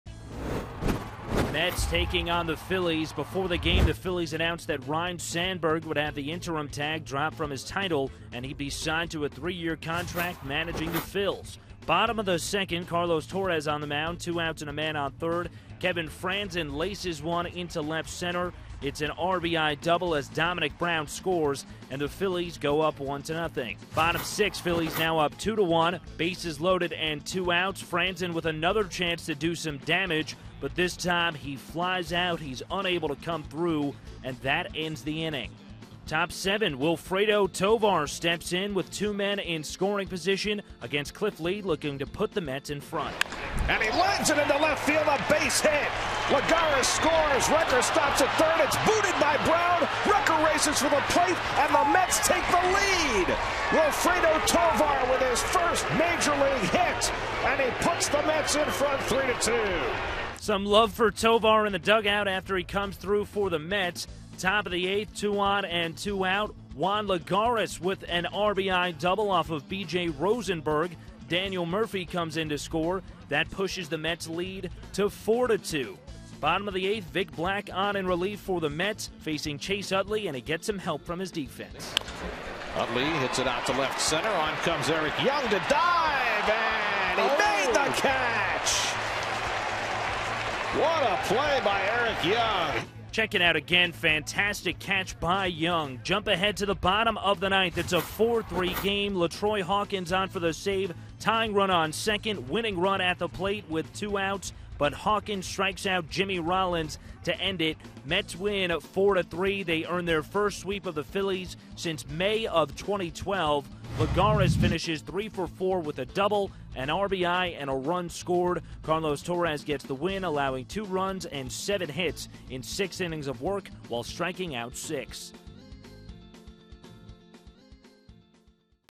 9/22/13: Tovar's first hit seals Mets' sweep of Phils
Daily Recap: Two runs scored on Wilfredo Tovar's first Major League hit, which proved to be the difference, sealing the Mets' 4-3 win